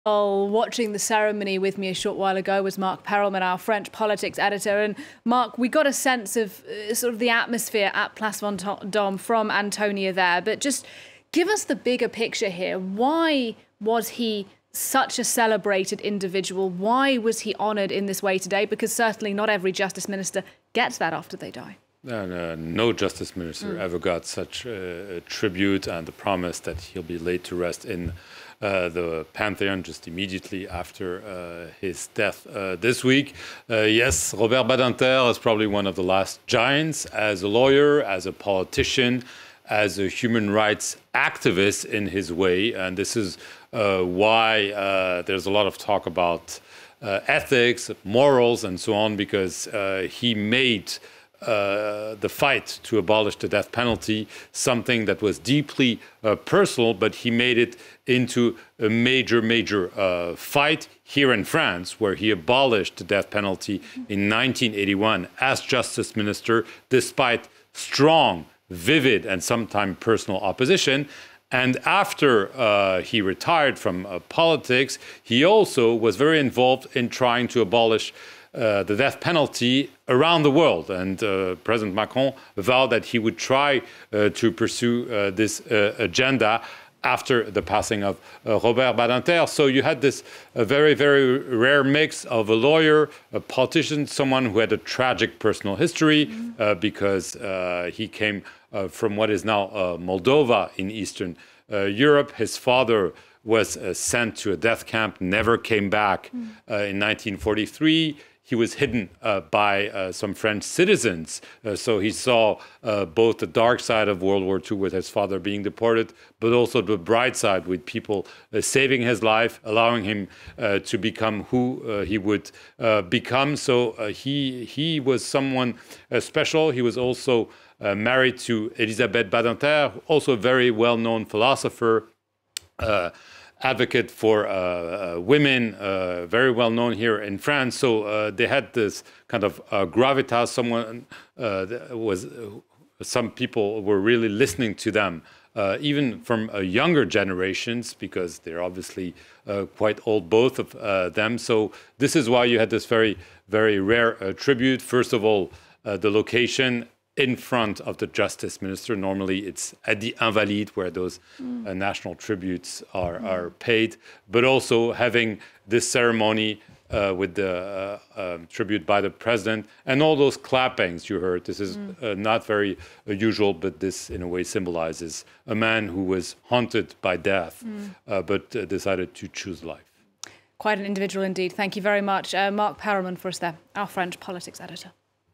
Robert Badinter: 'One of the giants' of French politics • FRANCE 24 English
Robert Badinter, the former French justice minister who was honoured at a ceremony in Paris on Wednesday following his death aged 95 last week, was a giant of French politics who battled to end the death penalty in France despite often strong and personal opposition in the country, says France 24's French politics editor Marc Perelman.